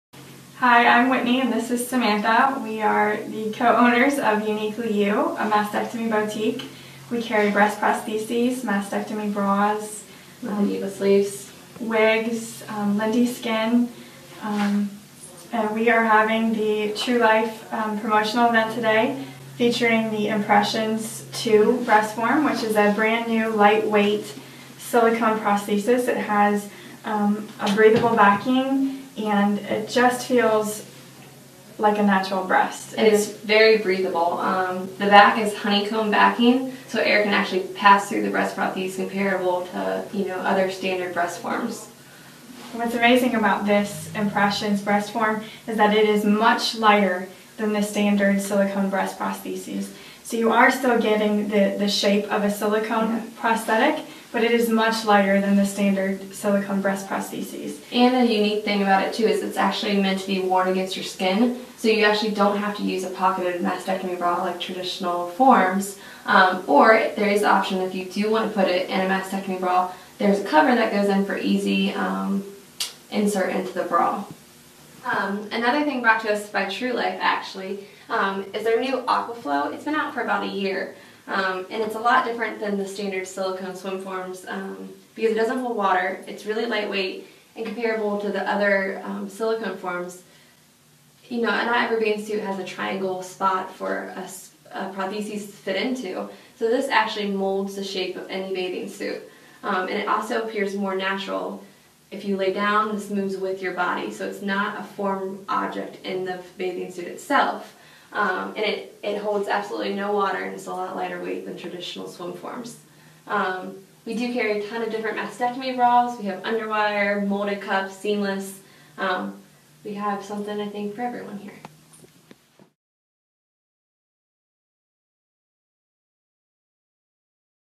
Uniquely You Mastectomy Boutique Shows Off their Lightweight Breast Prosthesis
Samantha and Whitney of Uniquely You Mastectomy Boutique recently had a special event to show off their Impressions lightweight breast form. They shared with us what makes this product so "unique."  They also told us about another one of their favorite breast prosthesis for swimmers. Two great products perfect for summer or living in warm weather states like Florida.